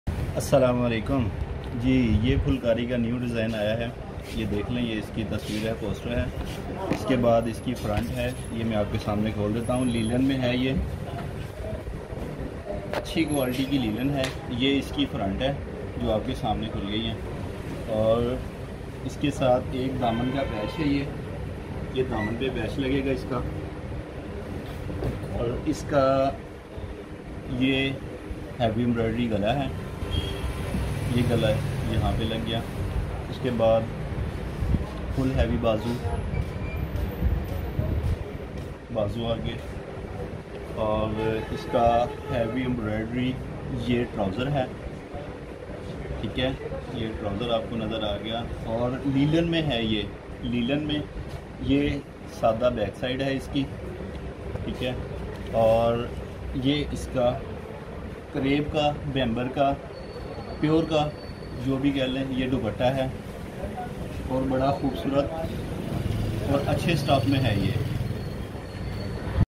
new phulkari design
PHUIKARI Kotal Linen collection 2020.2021
Fabric Kotal Linen
Brand Code HM 6685
Neck Heavy  emb patch
Front shirt full emb
Linen Kotal Sleeve fully Emb
Border heavy  emb  patch
Trouser  heavy  emb
Bamber Chiffon heavy emb  duppta